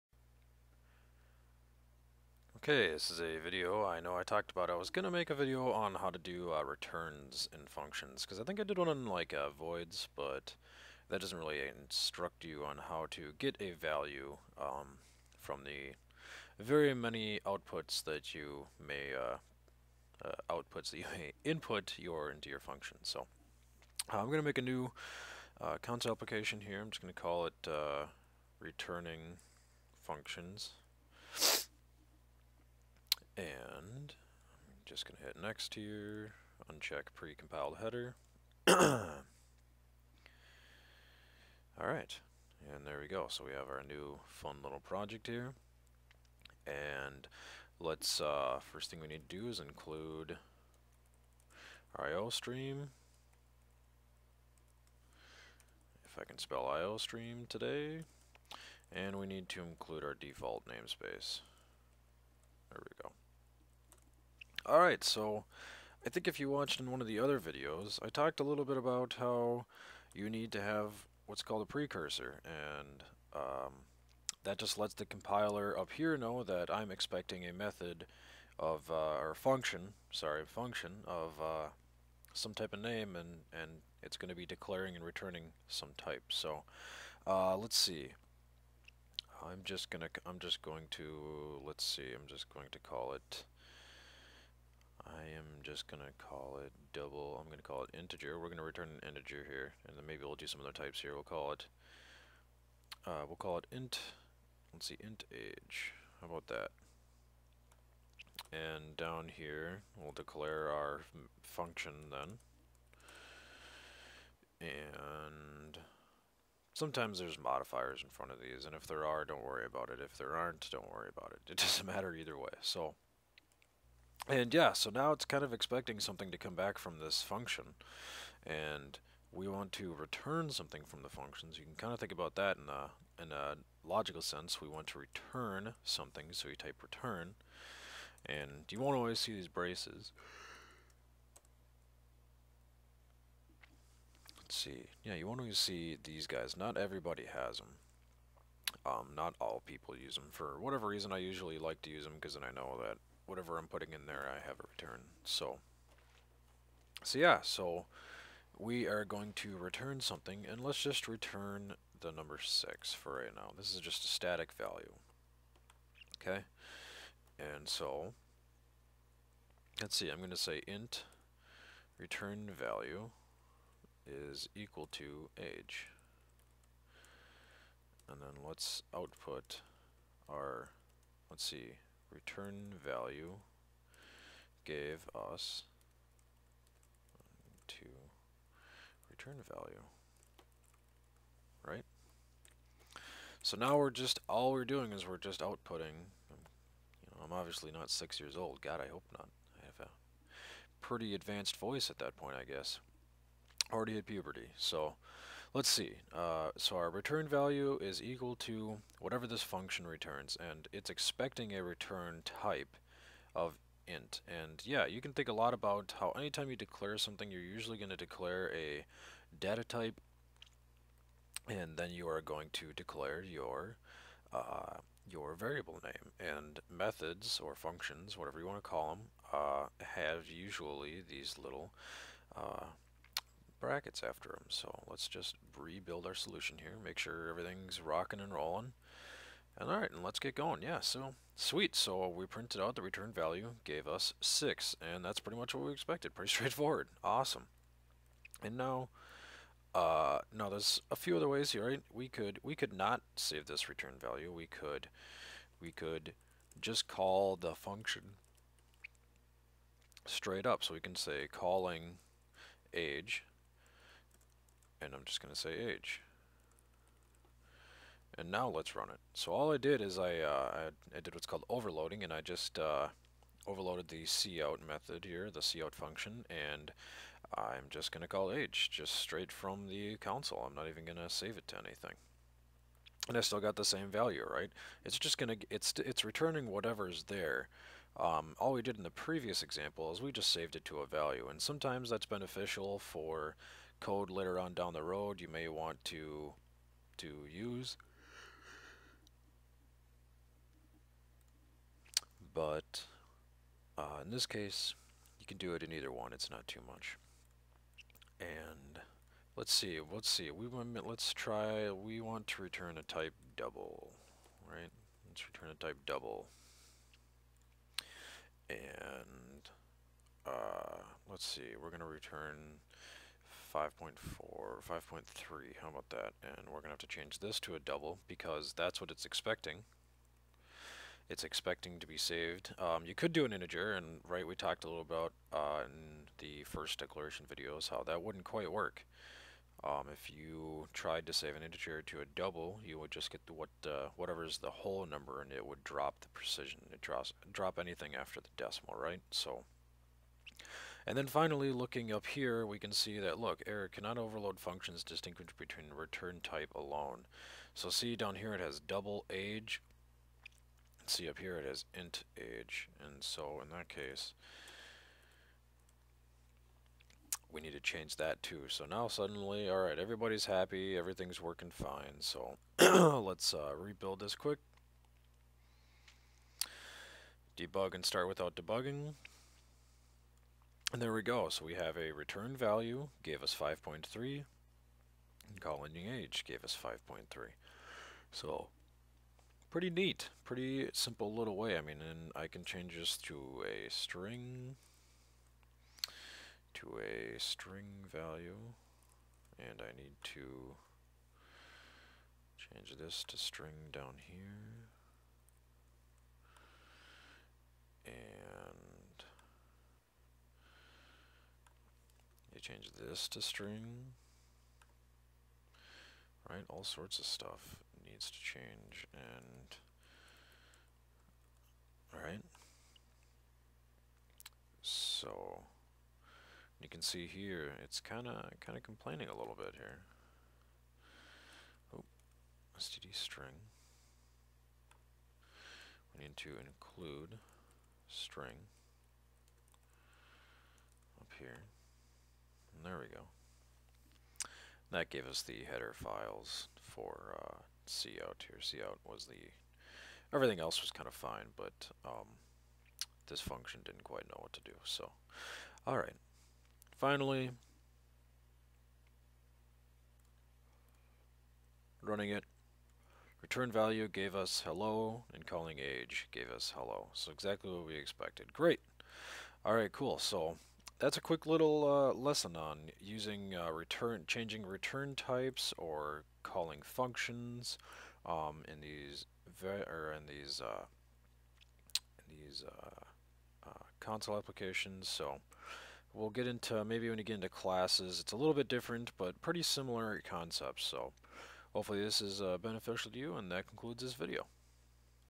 C++ Return functions
C++ console application on how to create functions/method to return a values. The video shows how to return type(s) int, double, and string